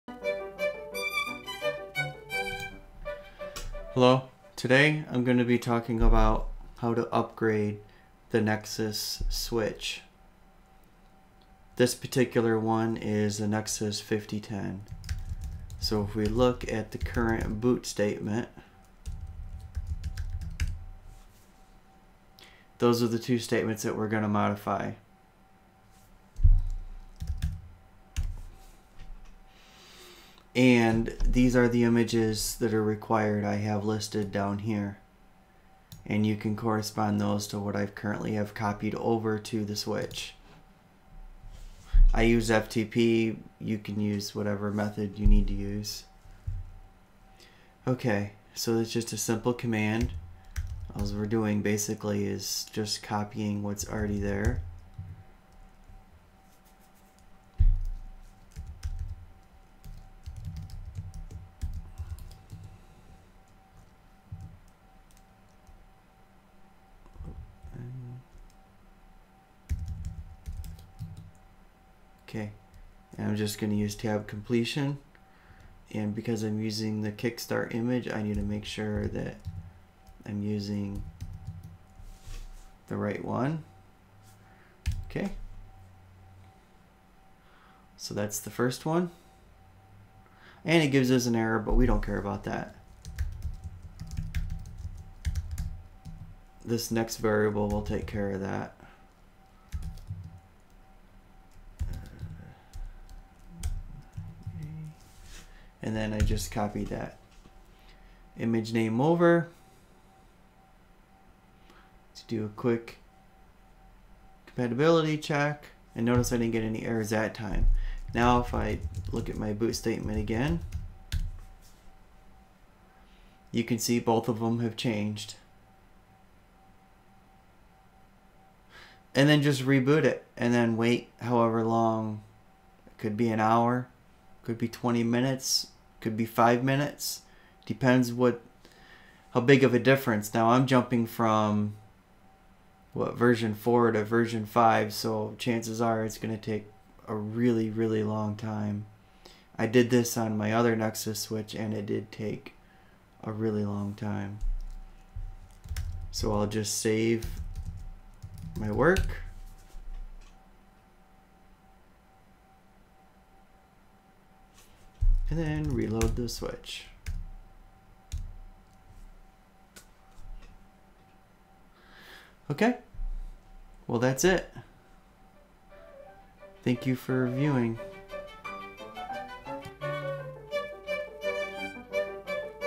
NX OS upgrade
How to upgrade a Cisco Nexus switch. I will be using a Nexus 5010.

Music is public domain from: musopen.org